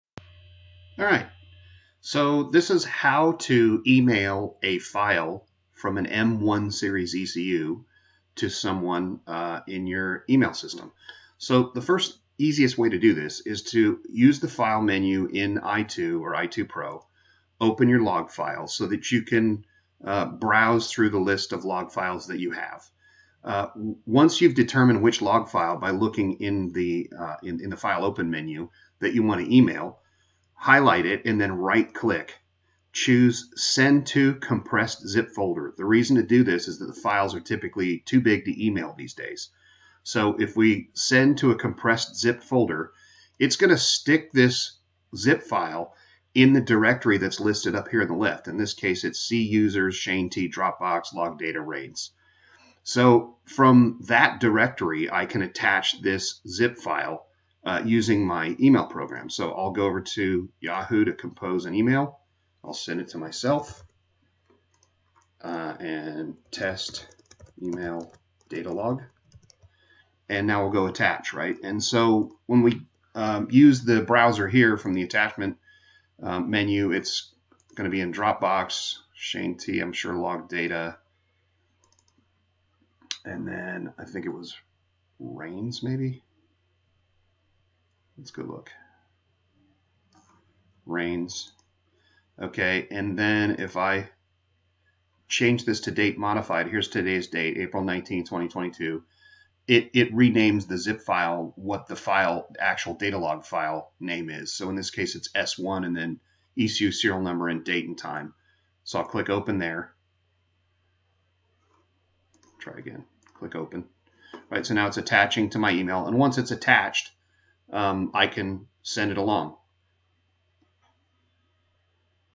Tuned By Shane T - How to - Email an M1 Log File from I2 - Factory Stock Showdown Firmware
In this video a method is described for attaching a MoTeC M1 Engine Management System Data Log File to your email to send to someone else.